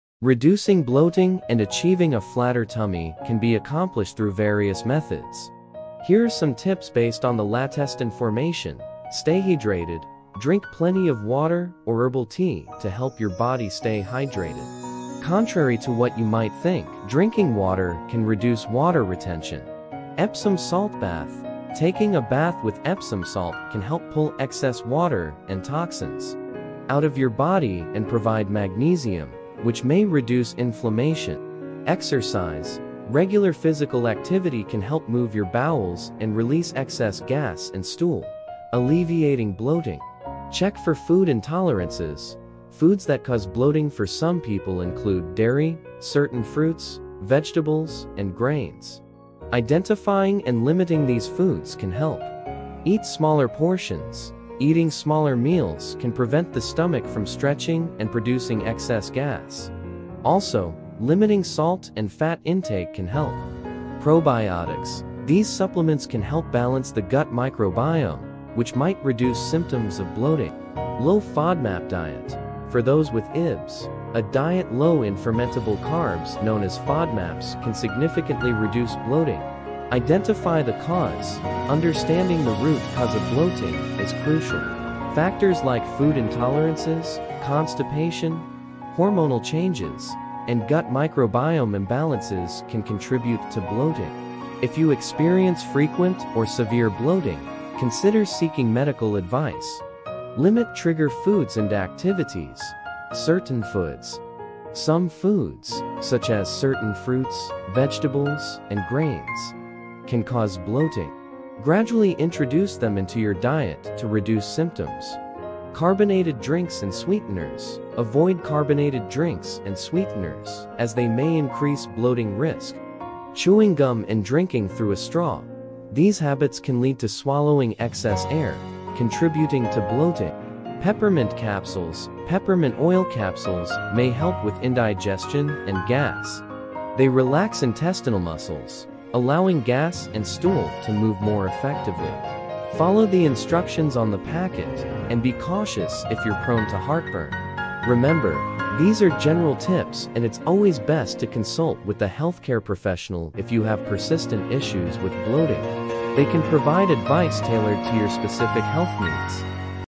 "How to Reduce Bloating and Flatten Your Tummy! bloating"healthyliving
🌟 Say Goodbye to Bloating and Hello to a Flatter Tummy! 🌟

Are you tired of feeling bloated and uncomfortable after every meal? It's time to reclaim your confidence and beat the bloat once and for all! In this video, we're sharing tried-and-true tips to help you reduce bloating and achieve a flatter, more comfortable tummy.

Join us as we dive into simple yet effective strategies to banish bloating from your life. From mindful eating techniques to herbal remedies, you'll discover a variety of methods to soothe your digestive system and restore balance to your gut.

Say farewell to that pesky belly bulge and hello to a happier, healthier you! Watch now and unlock the secrets to a flatter tummy and improved digestion.

🔗 Helpful Links:

10 Foods to Beat Bloating
Digestive Health Tips
Bloating Relief Exercises

📣 Call to Action:
Together, let's say goodbye to bloating and hello to a happier, healthier digestive system. 💪